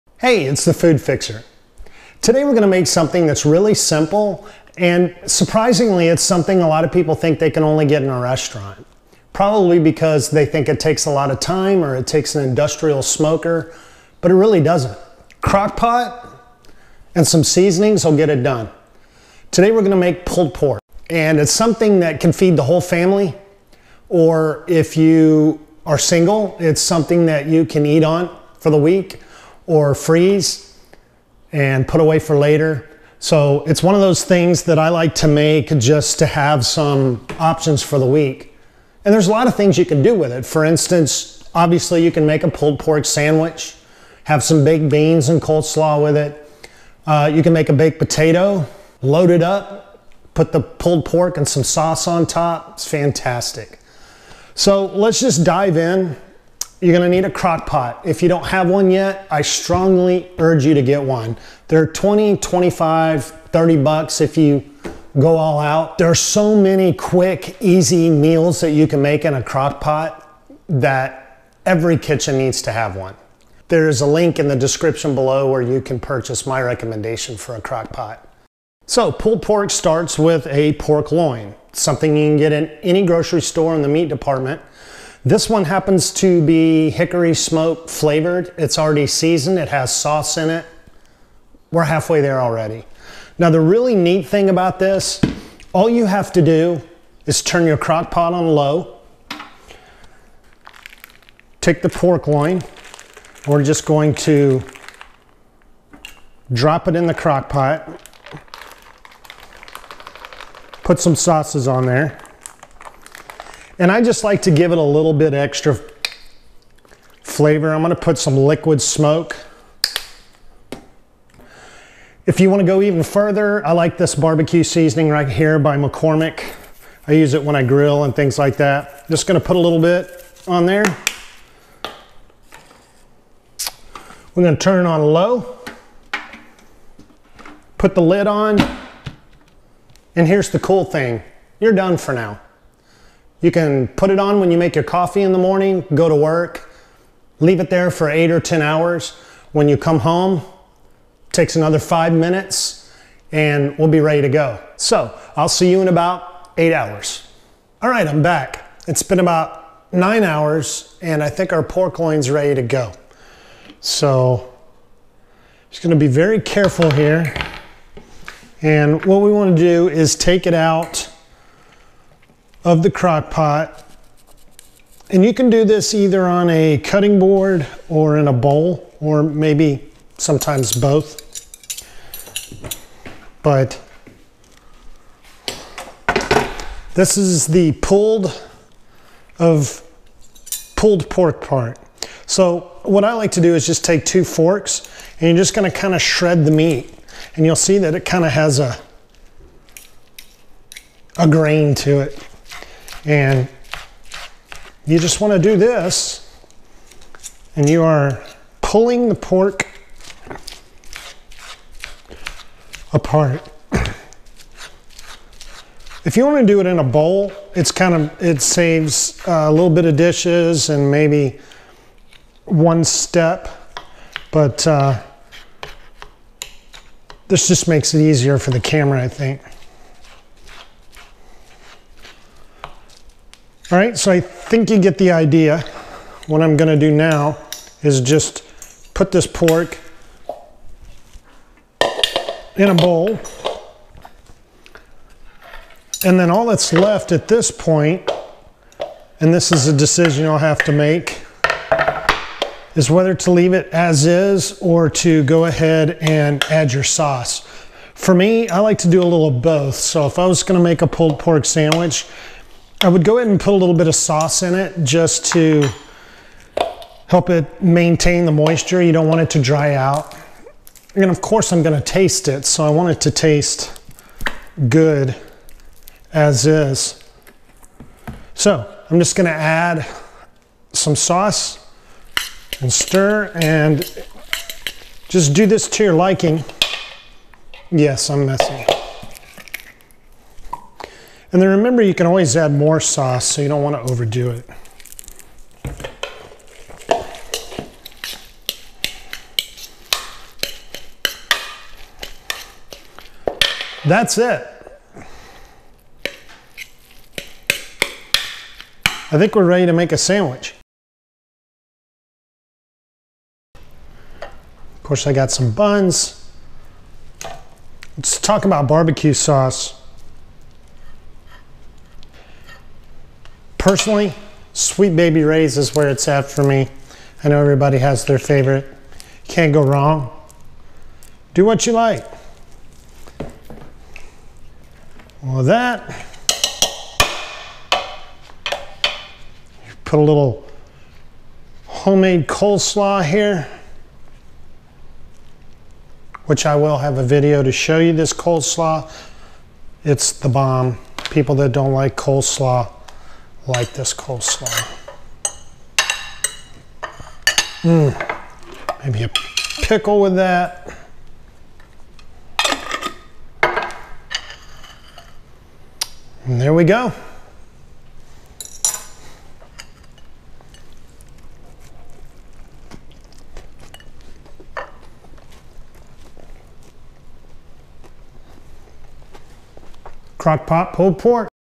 QUICK & EASY CROCK-POT PULLED PORK | THE FOOD FIXER
Fixing a nice, hot meal is simple. This video shows how anyone can make a delicious batch of pulled pork using only a slow cooker and two seasonings.